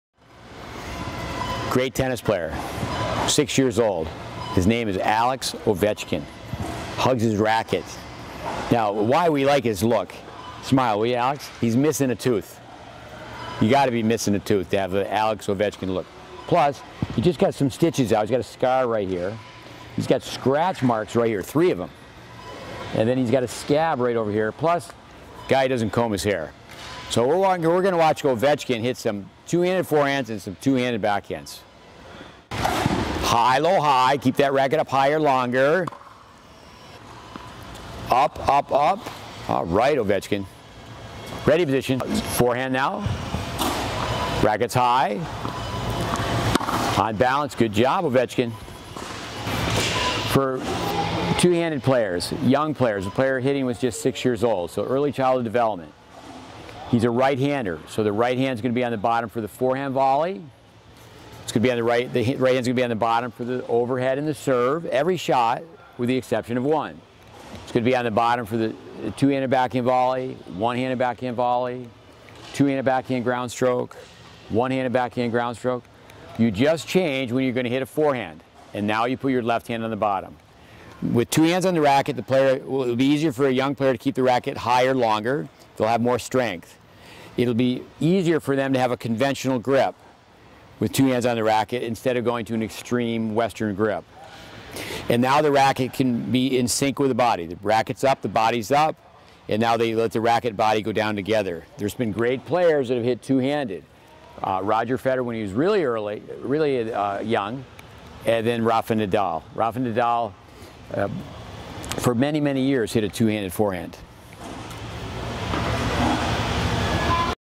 The Face of a Champion
Attached is a short film of a six year old boy. From a developmental standpoint, he is hitting a two-handed forehand. Technical advice is shared. The method will give the boy a better chance to hold the racquet high, have a shorter backswing, synchronize the swing with the body and avoid being programmed in the early childhood stage with a severe western grip.

Follow our project 'THE GREAT BASE" on Facebook